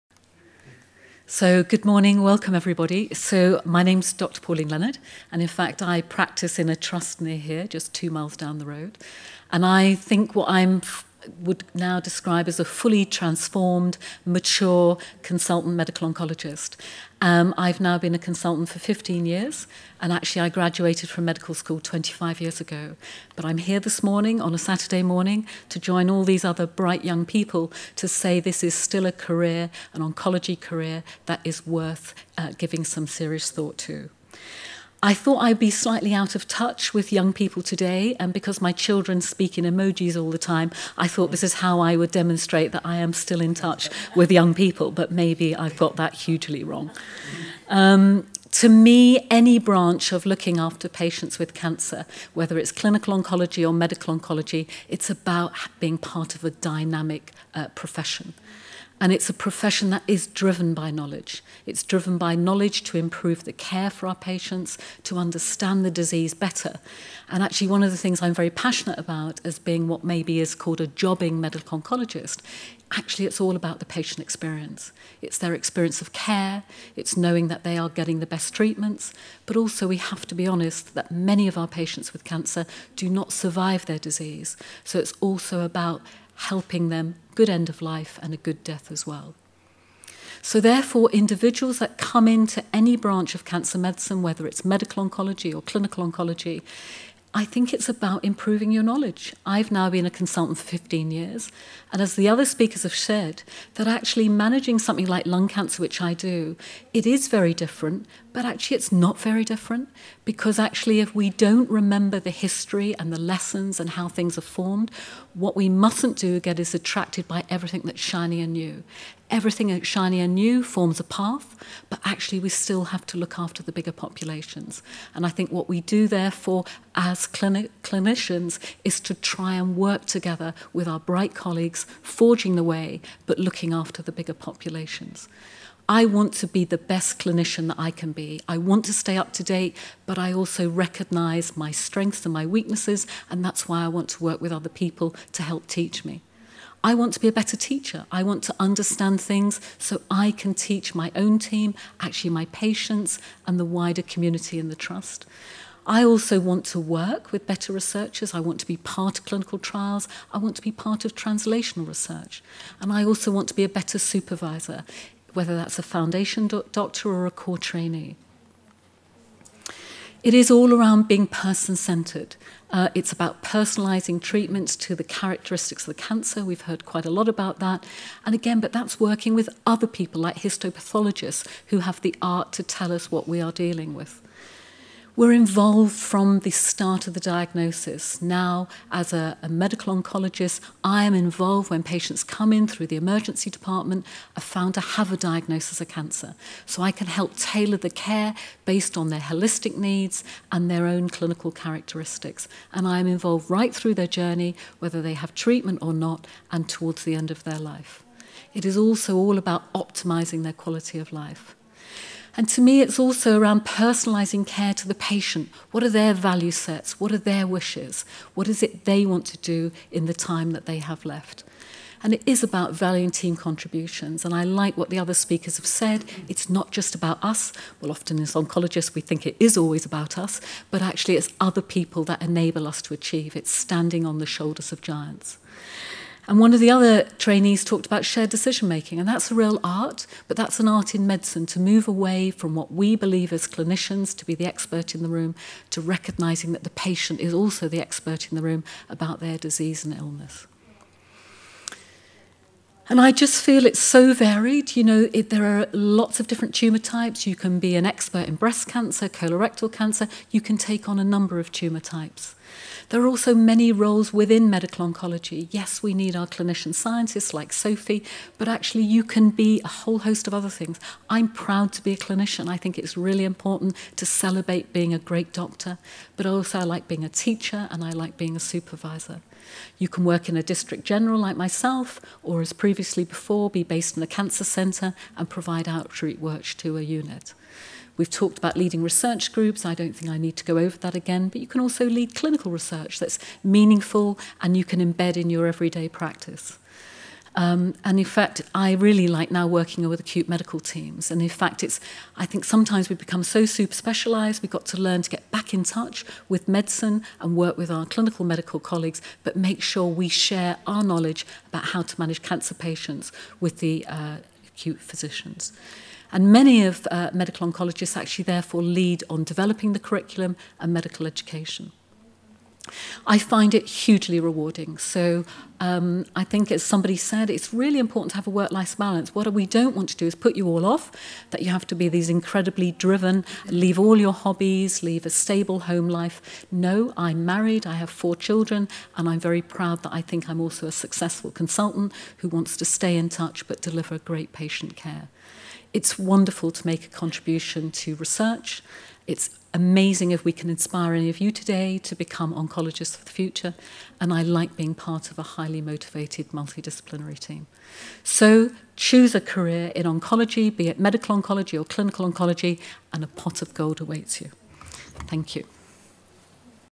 A career in oncology
Dr Pauline Leonard on what she loves about being a consultant medical oncologist. Find out more about a career in oncology: